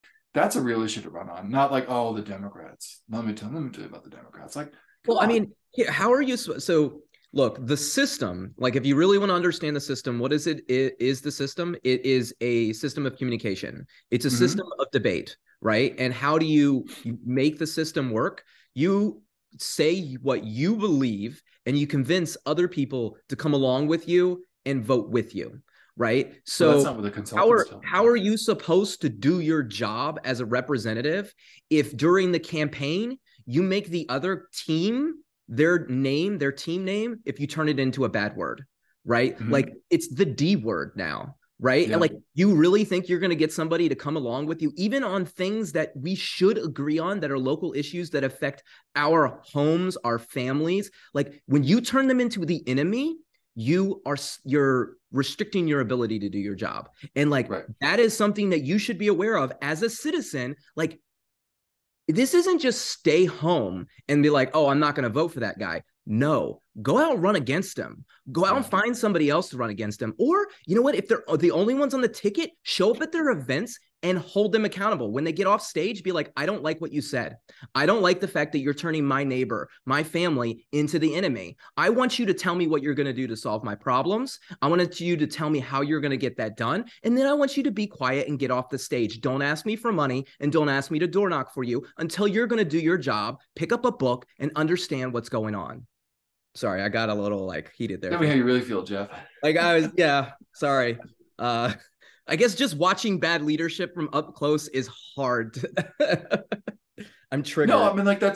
Triggered. Why do we allow politicians to make our neighbors the enemy?
Jeff gets a little heated, discussing our bad leadership in our union.